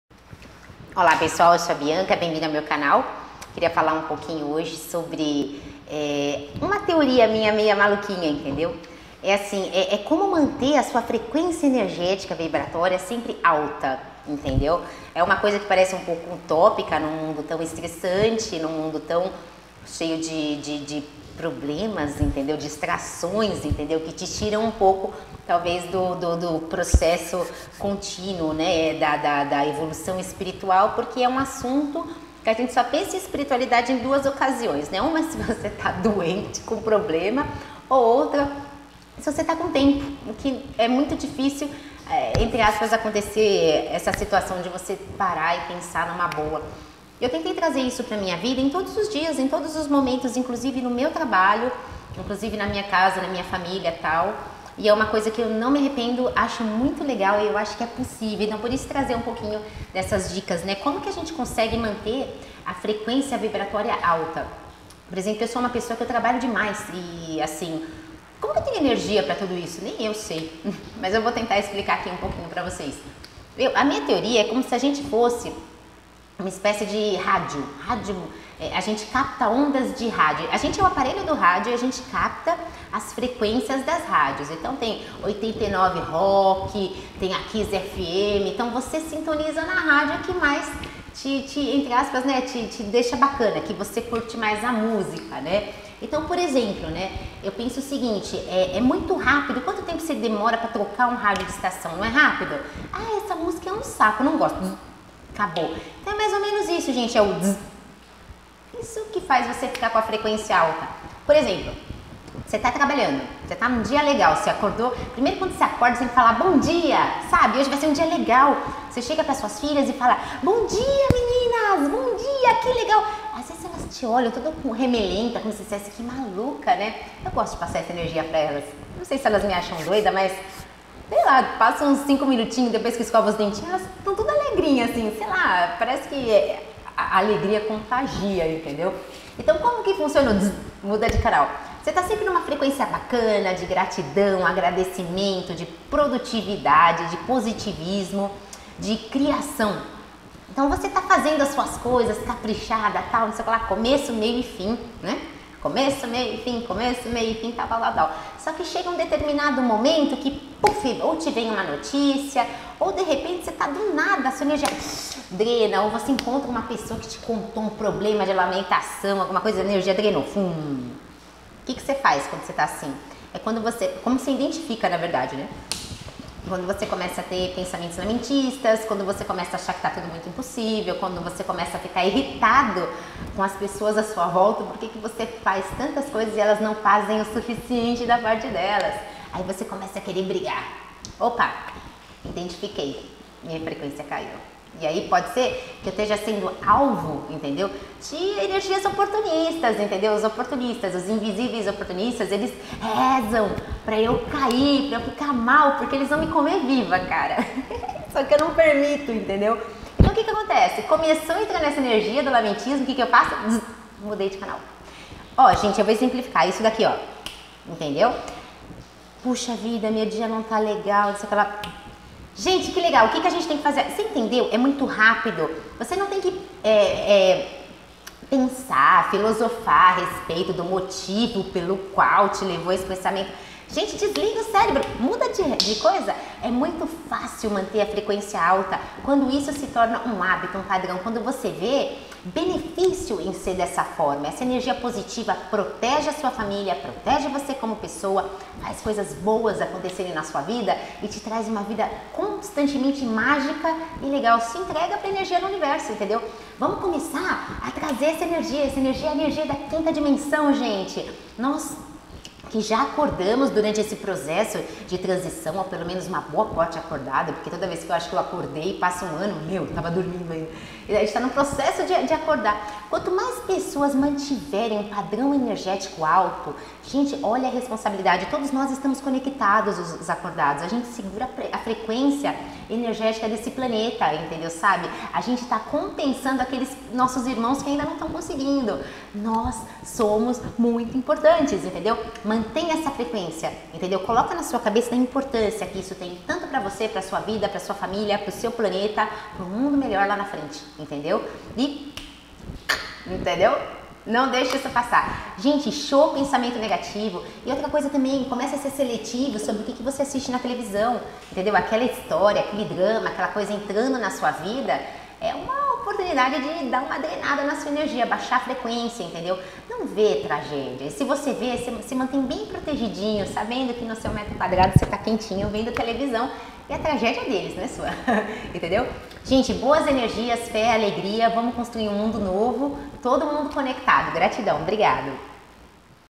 Como manter sua frequencia vibratoria sempre alta
Eu busquei muito essa paz, e fui sem referência, apenas com a certeza no coração de que eu não poderia morrer sem tentar kkk
E aqui fica as minhas dicas, meus relatos, de como é possível manter a frequencia energetica vibratória sempre alta, trazendo proteção e prosperidade, com criatividade e saúde para minhas crianças, gratidão universo !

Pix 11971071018